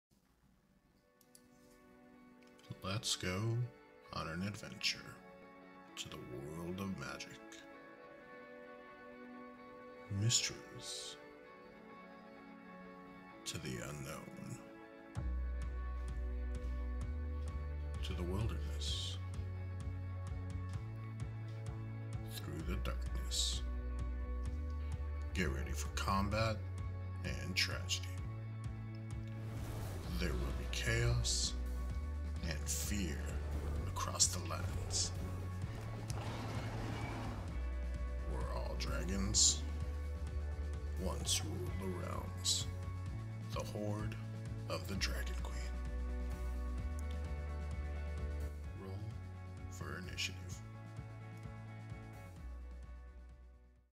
The Campaign Trailer: The Hoard of the Dragon Queen
This trailer is to get you hyped for the upcoming campaign that I will be taking part in!! It will be live on Twitch and uploaded on this channel for those who haven't seen it yet! Spread the word across the realms... a new story is going to appear...